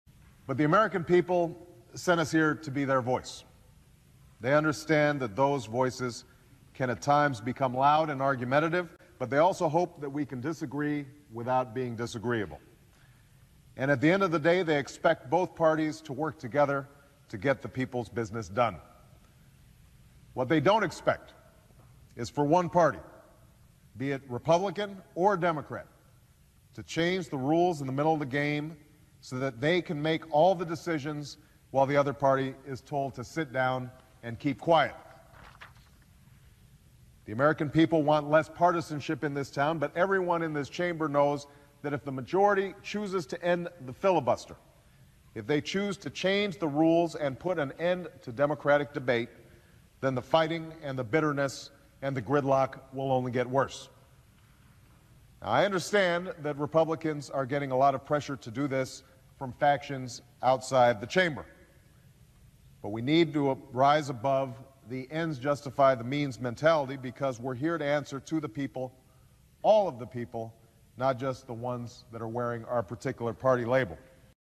FLASHBACK: Obama Defended The Filibuster In 2005 & Claimed Neither Party Should Abolish It
Back in 2005, Obama staunchly defended the filibuster when he was a Senator.

Obama went as far as stating that neither political party should “change the rules in the middle of the game so that they can make all the decisions while the other party is told to sit down and keep quiet.”

In Obama’s speech defending the filibuster, he also made a prediction: “If the majority chooses to end the filibuster, if they choose to change the rules and put an end to Democratic debate, then the fighting and the bitterness and the gridlock will only get worse.”

Since giving this speech, Obama has seemingly changed his views. He now refers to the filibuster as a “Jim Crow relic”.

What changed, Obama?